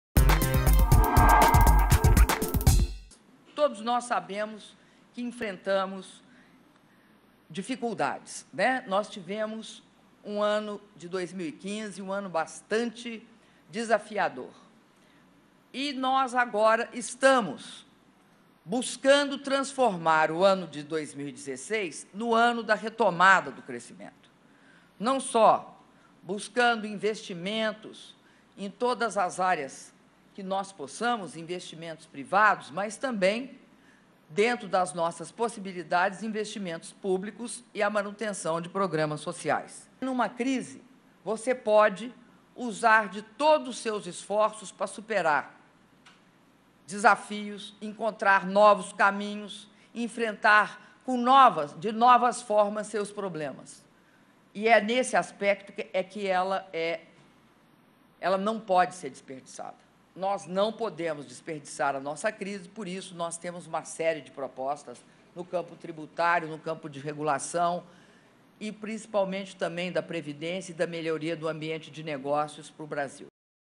Presidenta Dilma durante inauguração da fábrica da Ambev em Uberlândia
Presidenta Dilma Rousseff disse que 2015 foi um ano “bastante desafiador” e 2016 terá retomada do crescimento, durante a inauguração de uma fábrica da cervejaria da Ambev em Uberlândia.